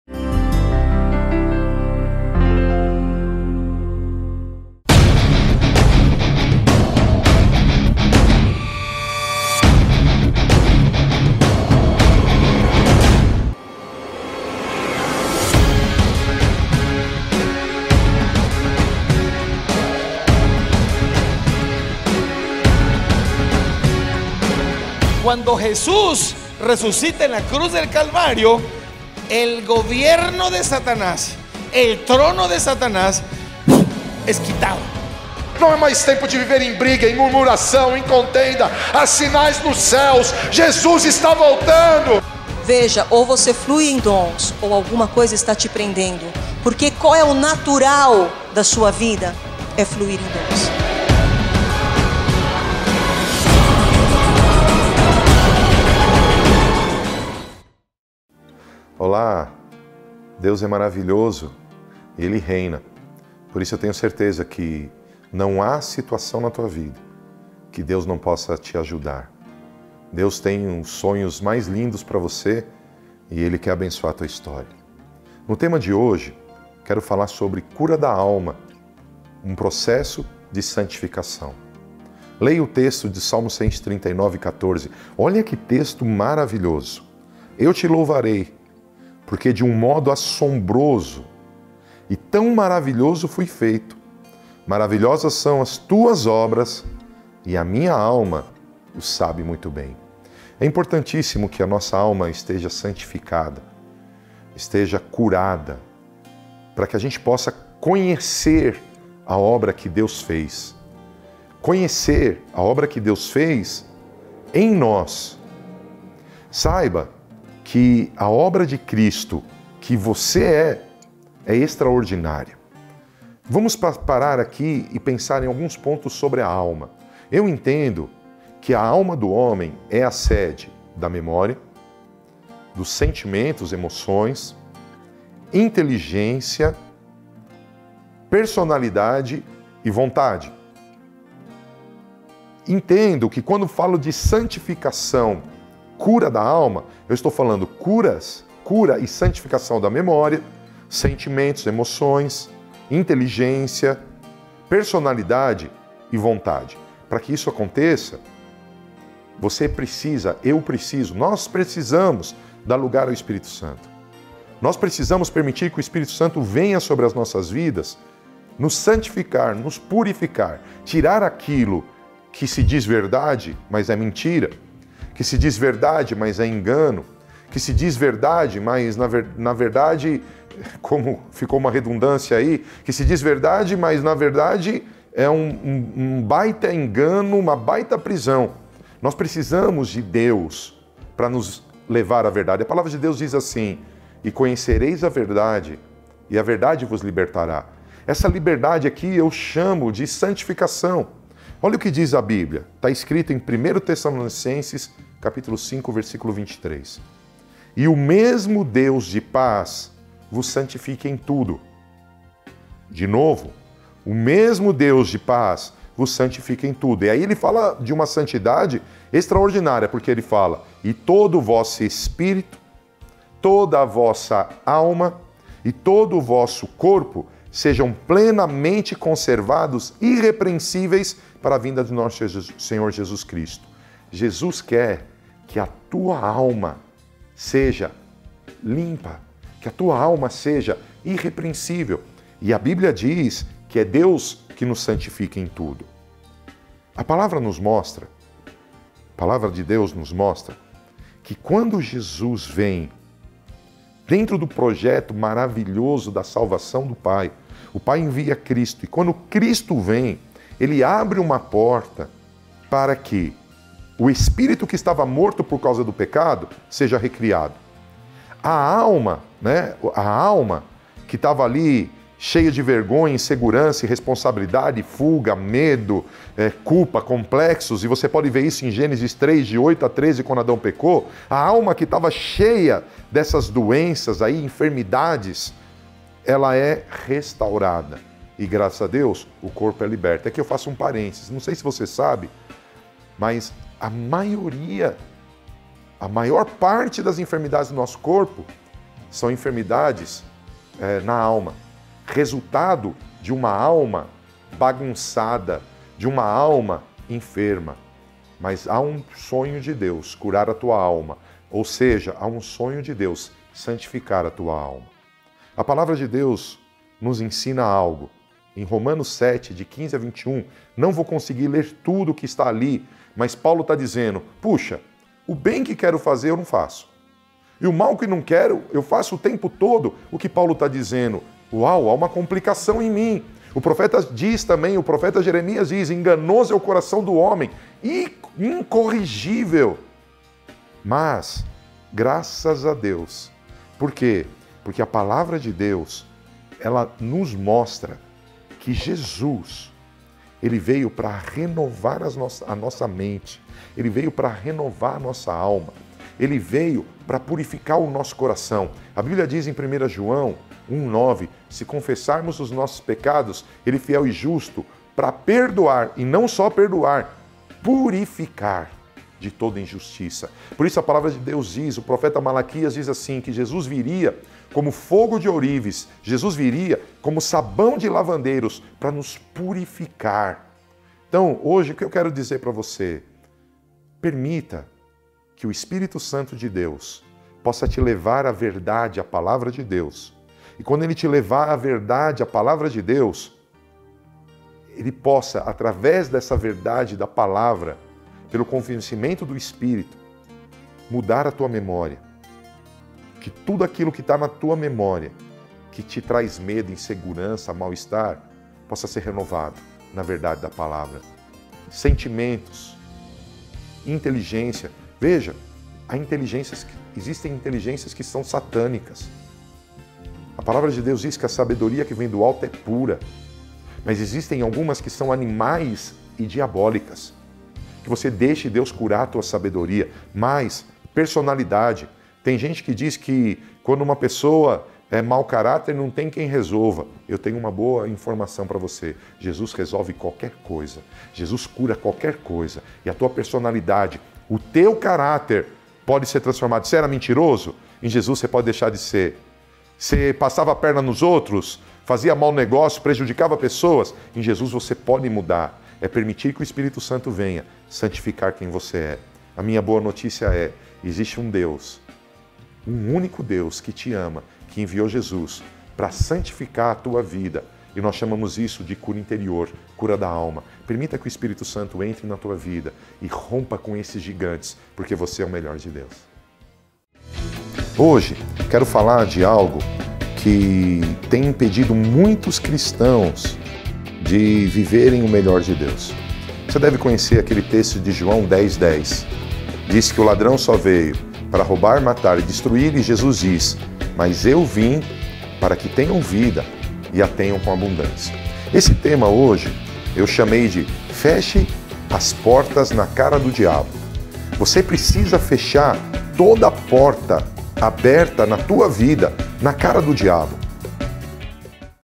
Palavra pra Você | Cura da alma
Aprenda com o pastor Paulo Tércio passos importantes para sua caminhada com Deus. O tema, "Cura da alma", será aprofundado em nossos próximos cultos, domingo às 9h e às 19h.

Venha estar conosco!

Não perca nosso próximo programa quinta-feira às 11h.

NDV, sempre conectado com a Presença de Deus!